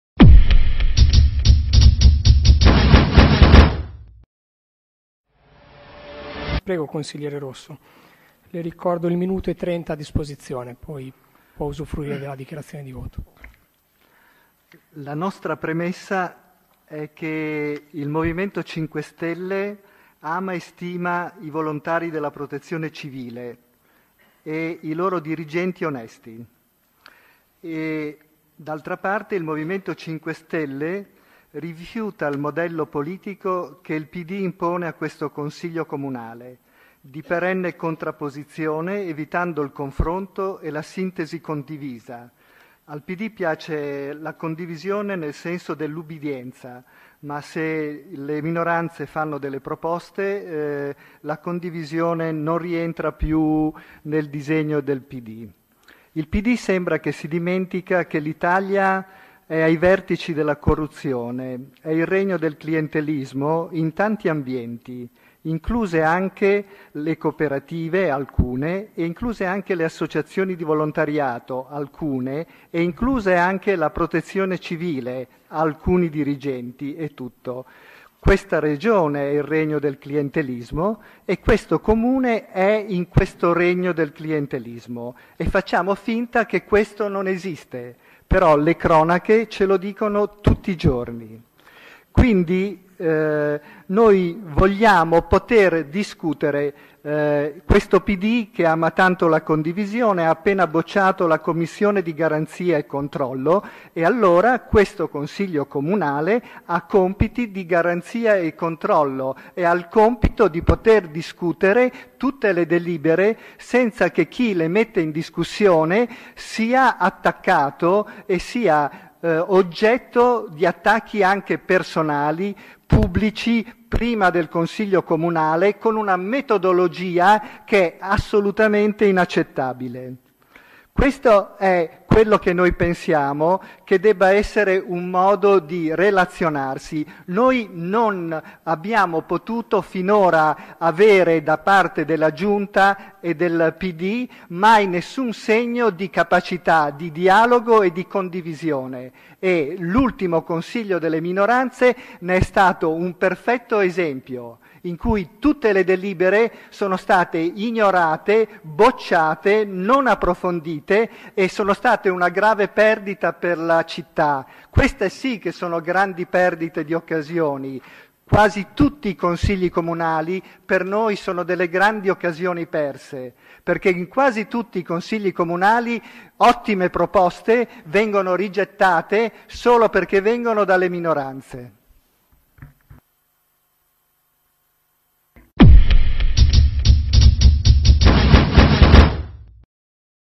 M5S - Consigliere G. Rosso P. Civile
P. Civile Discussione  - Consiglio Comunale del 18 Giugno

Proroga contratto di convenzione per la gestione degli alloggi ERP di proprietà comunale con l'ACER di Forlì-Cesena.

 Bilancio di previsione 2015 e pluriennale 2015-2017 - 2^ variazione.

 Modifiche ed integrazioni al regolamento del "Gruppo comunale Volontari di Protezione Civile" costituito con delibera di C.C. n.136 del 21/12/2011.

Affidamento del servizio per la gestione servizi socio-educativi ed educativo-didattici attraverso l'Azienda servizi alla persona Cesena - Valle Savio. Approvazione contratto.